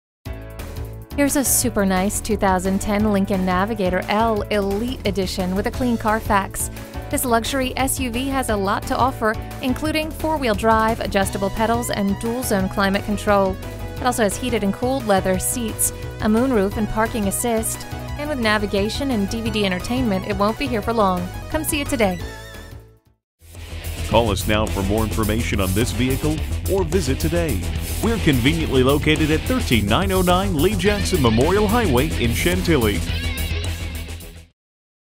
Used 2010 Lincoln Navigator L Chantilly VA Washington-DC, MD RXFC54971A - SOLD
Year: 2010
Make: Lincoln
Model: Navigator L
Trim: 4WD 4dr
Engine: 5.4 L 8 Cyl. Cyl.
Transmission: Automatic
Color: White Platinum Tri-coat Metallic
Interior: Camel
Mileage: 29501
Stock #: RXFC54971A
Clean Carfax!!! Navigation!!! DVD!!! L!!! Elite Edition! Super Nice!!! you have to see it!!!

Address:
13909 Lee Jackson Memorial Hwy